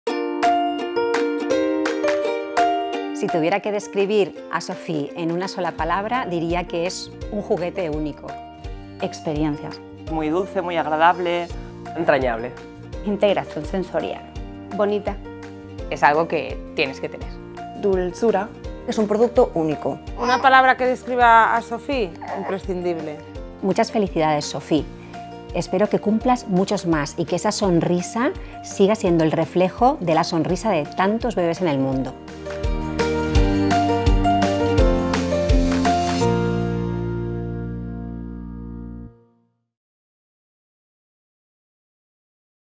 ¡Feliz Cumpleaños! 60 años con Sophie la girafe
El 25 de mayo hace 60 años nació Sophie la girafe. El juguete de  vanguardia que se ha convertido en un verdadero ícono atemporal y transgeneracional☺️

Desde BB Grenadine hemos querido hacerle una felicitación muy especial contando con profesionales y familias para que nos hablen y nos comenten su experiencia con Sophie. Este es el primer video de muchos más que seguro que te van a encantar. ¡Estate atento porque cada mes hablaremos con uno de ellos!

Porque Sophie la girafe cruza épocas y fronteras sin envejecer, y con un mismo deseo: calmar la infancia de muchas generaciones por venir.

¡Feliz Cumpleaños Sophie!😉🎈